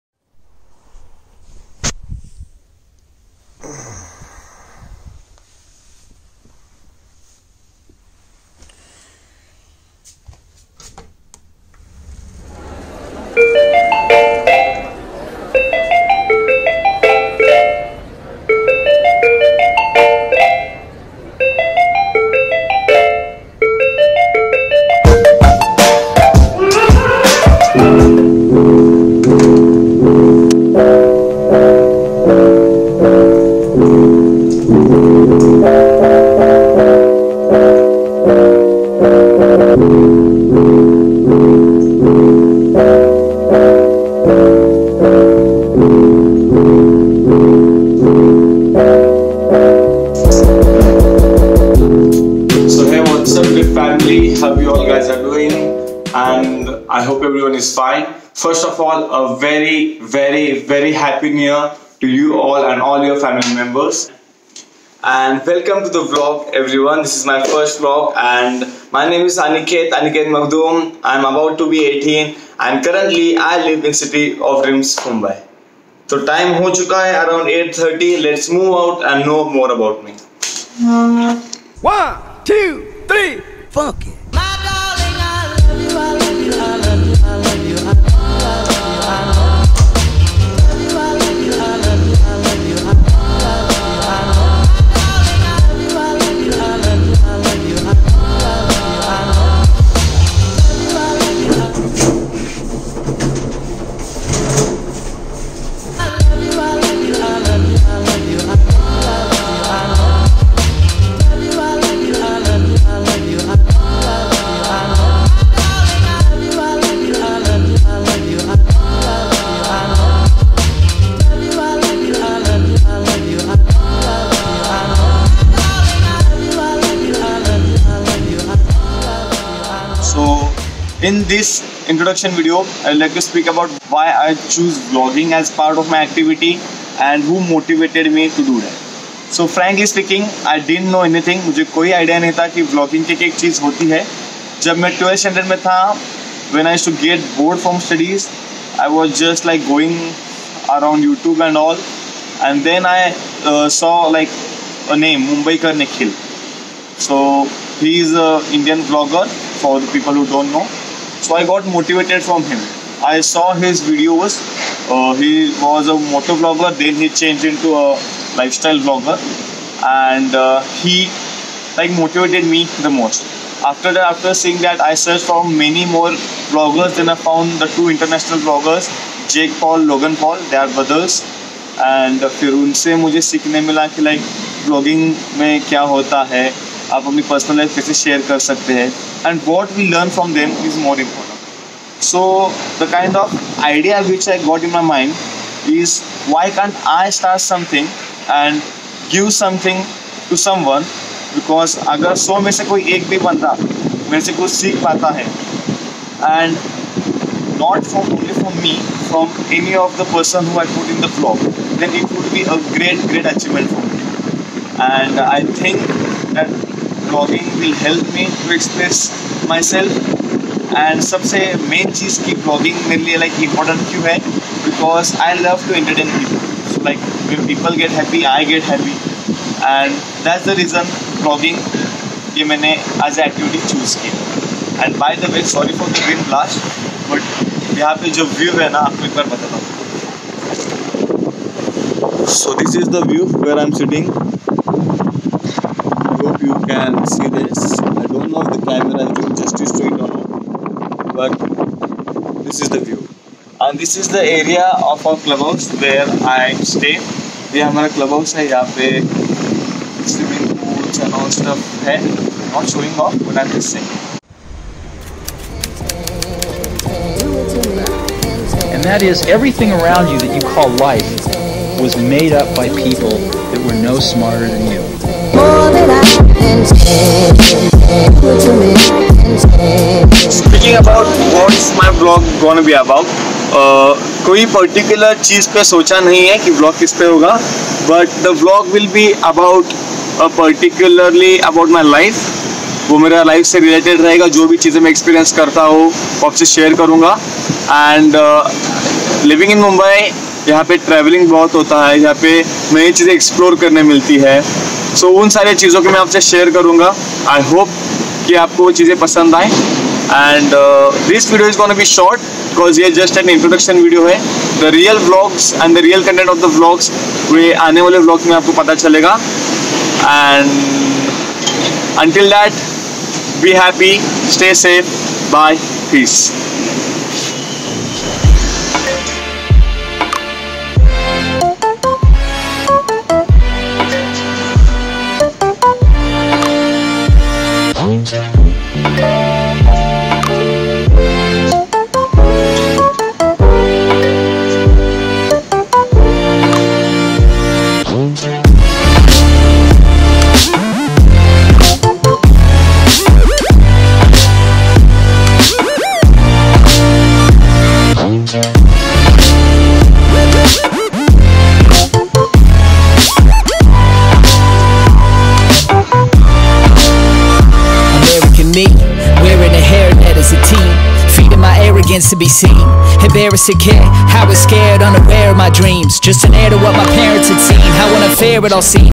INTRO to Aniket Vlogs.
Hello youtube fam , I am beginning a new journey on youtube with you guys. I would love all your support to the channel and the videos.

PEACE OUT, YOLO